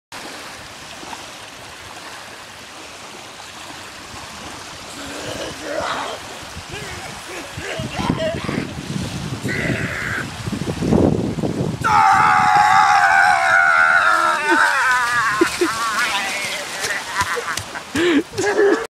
UNDERWATER FEMA DEATH CAMP
I found a FEMA Death camp underwater with Jesse the Body Ventura